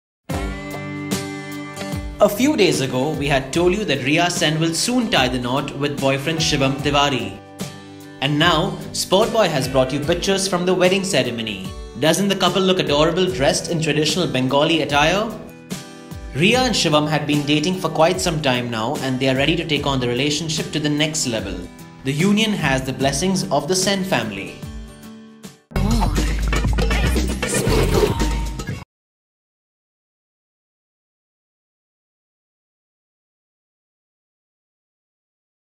Riya Sen MARRIES Shivam Tewari In A Hush-Hush Ceremony | SpotboyE
Yes, Bong bombshell Riya Sen has tied the knot with long-time boyfriend Shivam Tewari in a private ceremony.